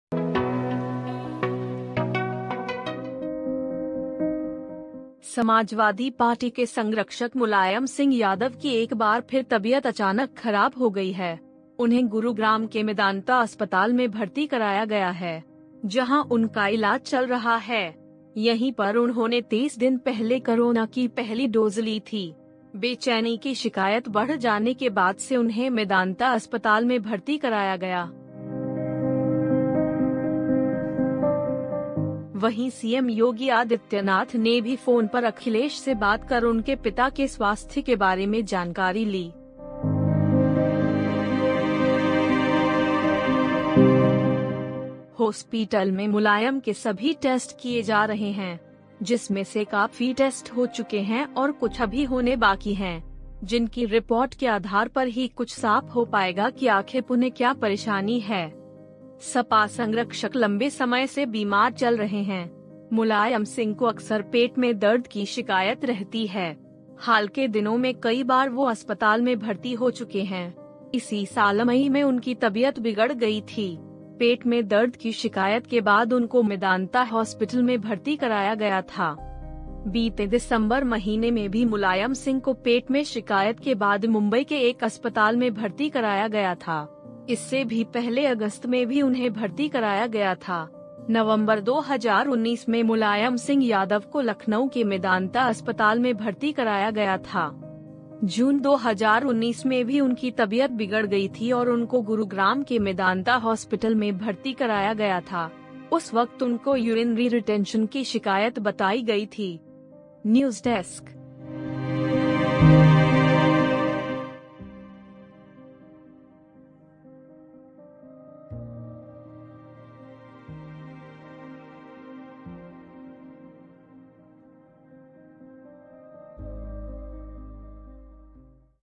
Gurugram के Mednata Hospital में भर्ती हुए Mulayan Singh Yadav, यहीं पर ली थी कोरोना की पहली डोज़
समाजवादी पार्टी के संरक्षक मुलायम सिंह यादव की एक बार फिर तबीयत अचानक खराब हो गई है। उन्हें गुरुग्राम के मेदांता अस्पताल में भर्ती कराया गया है। जहां उनका इलाज चल रहा है। यहीं पर उन्होंने तेईस दिन पहले कोरोना की पहली डोज़ ली थी। बेचैनी की शिकायत बढ़ जाने के बाद से उन्हें मेदांता अस्पताल में भर्ती कराया गया।